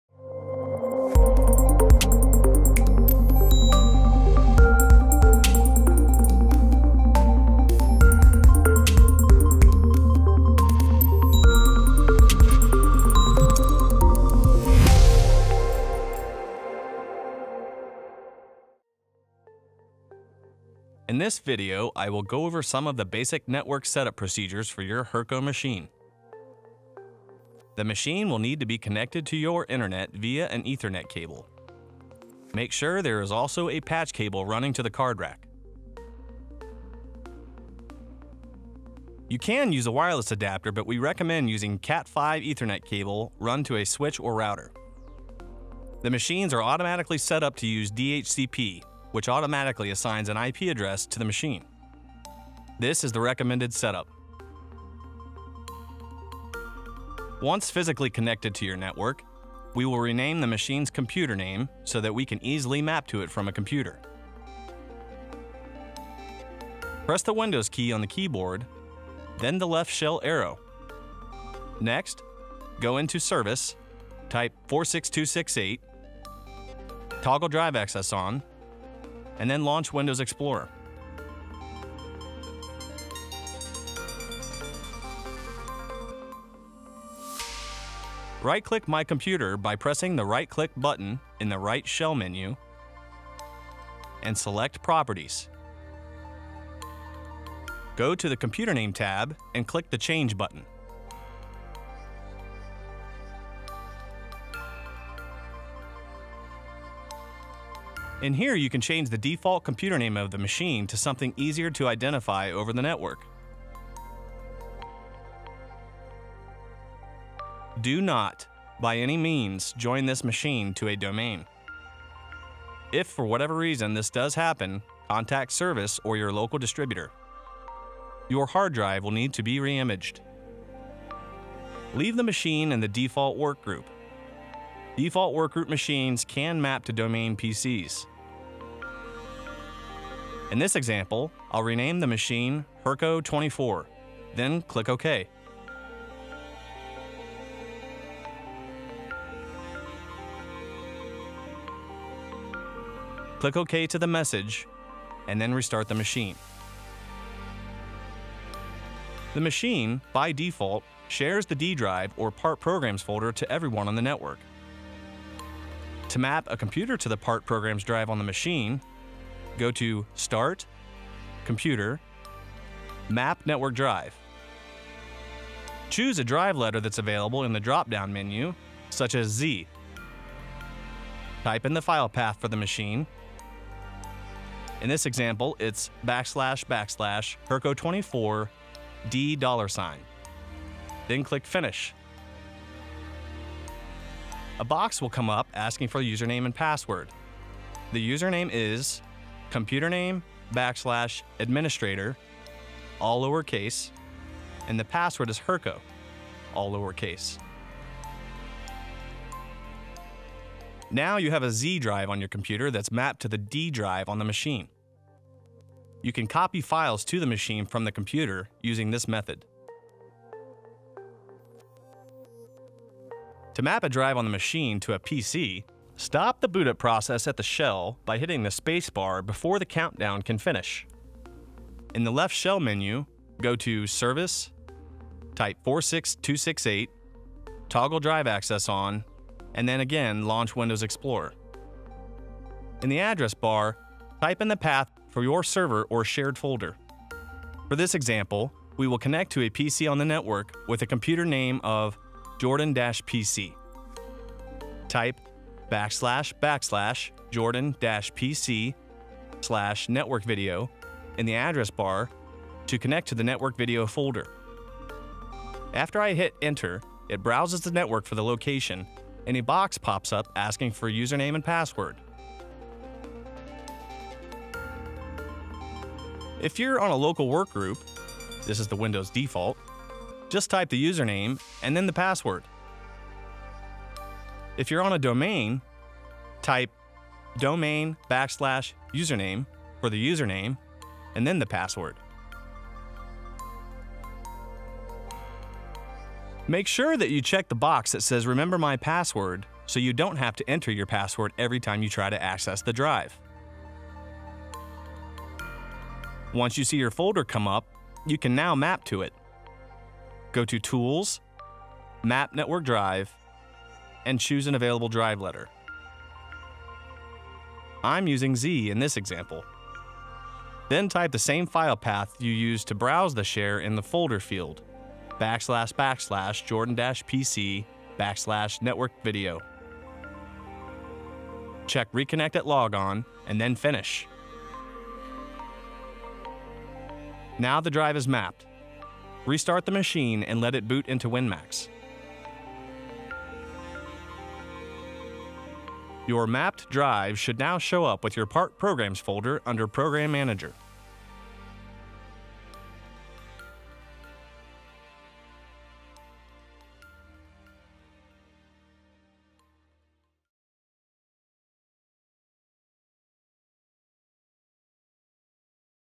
Hurco Network Setup
• In this video, I will go over some of the basic network setup procedures for your Hurco machine.
The machine will need to be connected to your internet via an Ethernet cable.  Make sure there is also a patch cable running to the card rack.
• You can use a wireless adapter but we recommend using Cat5 Ethernet Cable run to a switch or router.
• The machines are automatically setup to use DHCP which automatically assigns an IP address to the machine.  This is the recommended setup.

Mapping from a PC to the machine
• Once physically connected to your network, we will rename the machine’s computer name so that we can easily map to it from a computer.
• Press the Windows Key on the keyboard, then the left shell arrow
• Next, go into service, type 46268, toggle drive access on, then Windows Explorer
• Right click My Computer by pressing the right click button in the right shell menu and select properties
• Go to the computer name tab and click the change button
• In here you can change the default computer name of the machine to something easier to identify over the network
• DO NOT BY ANY MEANS JOIN THIS MACHINE TO A DOMAIN.  IF FOR WHATEVER REASON THIS DOES HAPPEN, CONTACT SERVICE OR YOUR LOCAL DISTRIBUTOR.  Your hard drive will need to be reimaged.
• Leave the machine in the default workgroup.  Default workgroup machines can map to domain PCs.
• In this example, I will rename the machine HURCO24, then click ok.  Click ok to the message and then restart the machine.
• Once again, stop the bootup process at the shell
• The machine, by default, shares the D: drive or Part Programs folder to everyone on the network
• To map a computer to the Part Programs drive on the machine
• Go to start, computer, map network drive.  Choose a drive letter that’s available such as Z:
• Type in the file path for your machine.  In this example it’s \\hurco24\d$ then click finish
• The username is computername\administrator and the password is hurco all lower case
• Example:  hurco24\administrator
Password:  hurco
• Now you have a Z: on your computer that’s mapped to the D: drive on the machine.  You can copy files
to the machine from the computer using this method.

Mapping from a machine to a PC
• To map a drive on the machine to a PC, start up the machine and stop at the shell In the left shell menu,
• go to service, type 46268, toggle drive access on and then windows explorer
• In the address bar, type in the path for your server or shared folder
• For this example we will connect to a PC on the network with a computer name of “jordan-PC”
• Type \\jordan-pc\networkvideo in the address bar to connect to the network video folder
• After I hit enter, it browses the network for the location and a box pops up asking for username and password.
• If you’re on a local workgroup (This is the windows default), just type the username and then the password
• If you are on a domain, type domain\username for the username and then the password
• MAKE SURE to check the box that says “remember my password” so you don’t have to enter your password everytime you try to access this drive.
• Once you see your folder come up, you can now map it.
• Go to tools, map network drive, and choose an available drive letter.  I’m using Z: in this example
• Then type the same file path you used to browse the share in the folder field \\jordan-pc\networkvideo
• Check reconnect at logon and then finish.  Now the drive is mapped, restart the machine and let it boot into WinMax
• Your drive should show up with your Part Programs folder under Program Manager